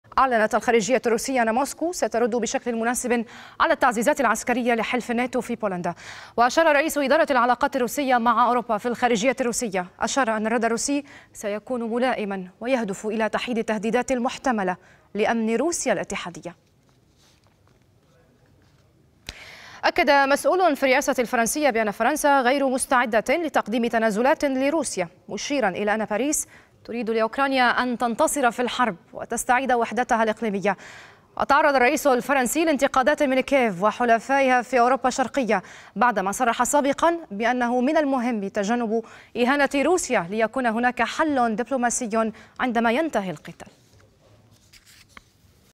فرنسا "ليست مستعدة لتقديم تنازلات" لروسيا
أكد مسؤول في الرئاسة الفرنسية بأن فرنسا غير مستعدة لتقديم تنازلات لروسيا،
مشيرا إلى أن باريس تريد لأوكرانيا أن تنتصر في الحرب وتستعيد وحدتها الإقليمية.
وتعرض الرئيس الفرنسي، إيمانويل ماكرون، لانتقادات من كييف وحلفائها في أوروبا الشرقية بعدما صرح سابقا بأن من المهم تجنبَ إهانة روسيا ليكون هناك حل دبلوماسي عندما ينتهي القتال.